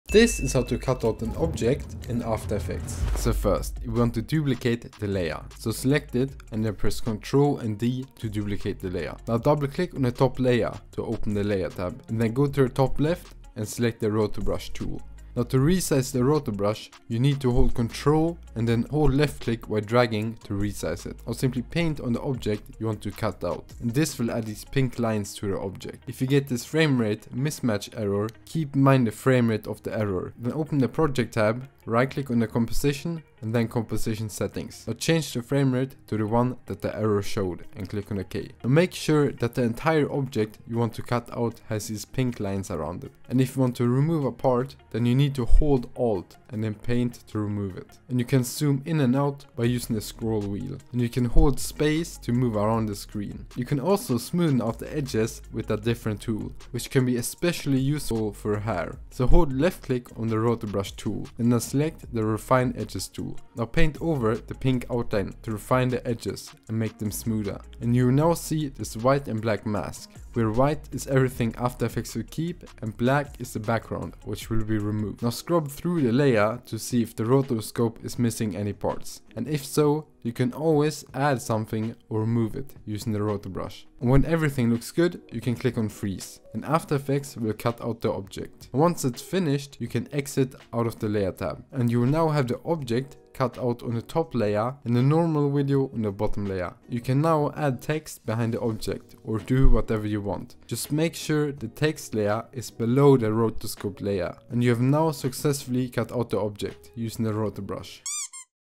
How To Rotoscope in After Effects
How To Cut Out a Person in After Effects in after effects

Chapters:
00:00 - Cut out object tutorial
00:13 - Roto Brush
00:26 - Frame Rate Mismatch Error Fix
00:41 - Rotoscoping
00:59 - Refine Edge tool
01:05 - Rotoscoping Hair
01:25 - Cut out person
01:37 - Adding Text behind object